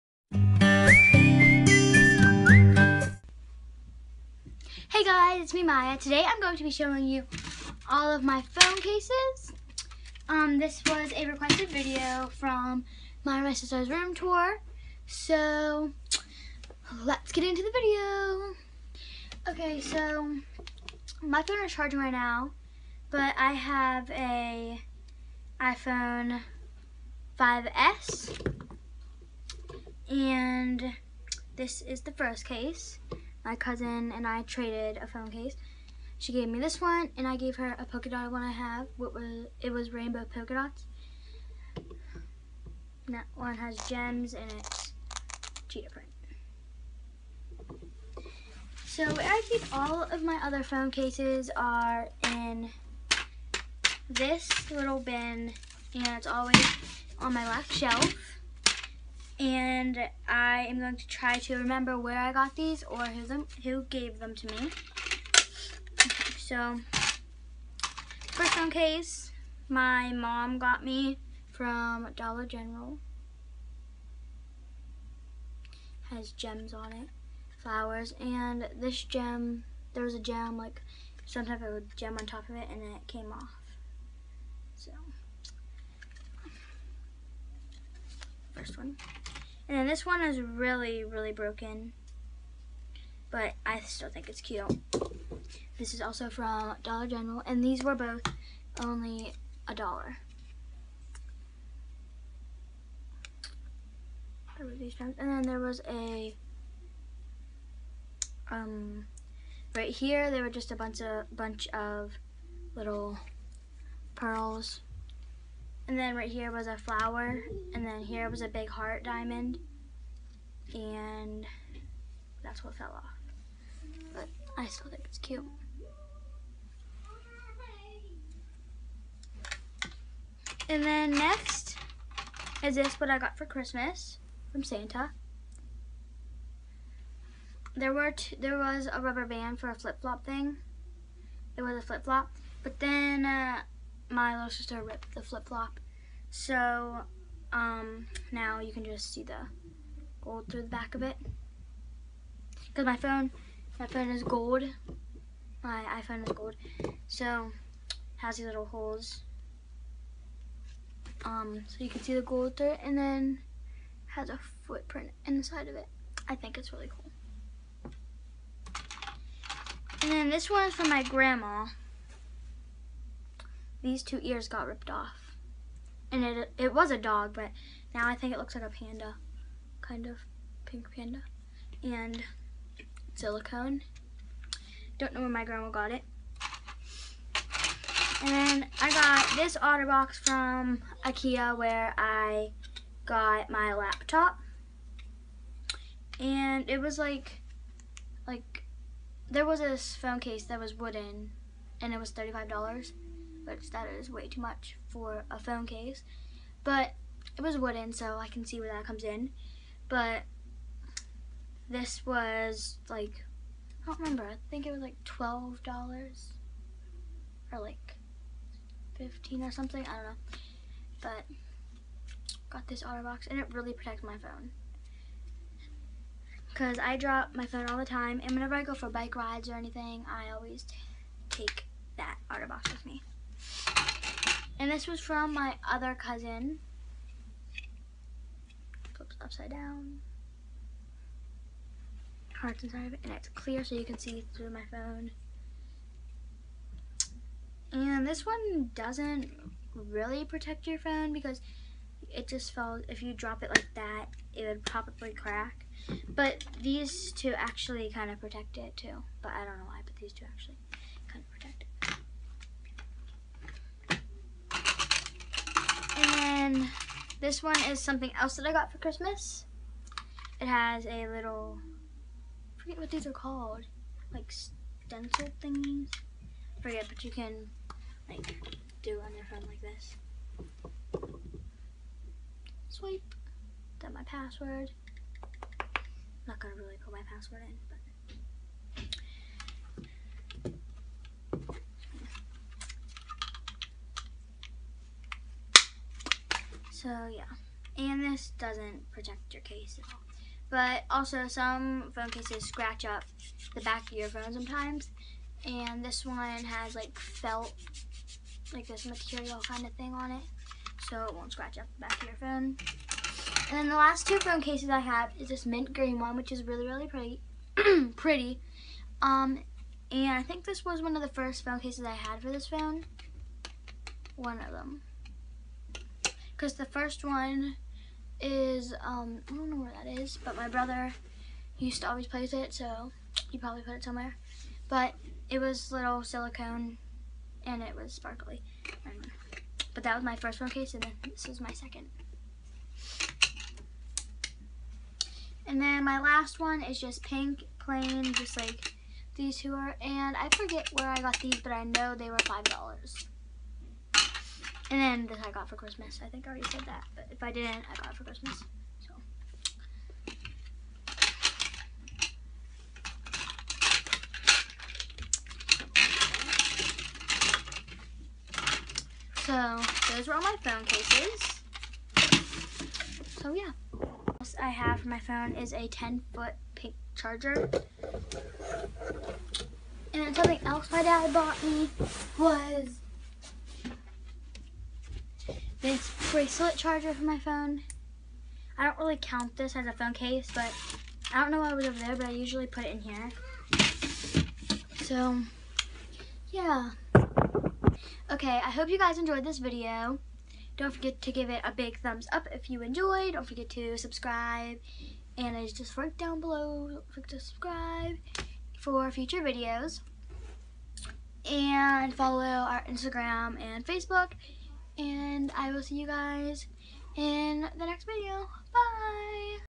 MYA'S iPHONE 5S PHONE CASE COLLECTION
VIDEO REQUEST; Mya shares her iPhone 5S phone cases. A few subscribers have asked for this video so here it is! Any other video requests please leave them in the comments! Xox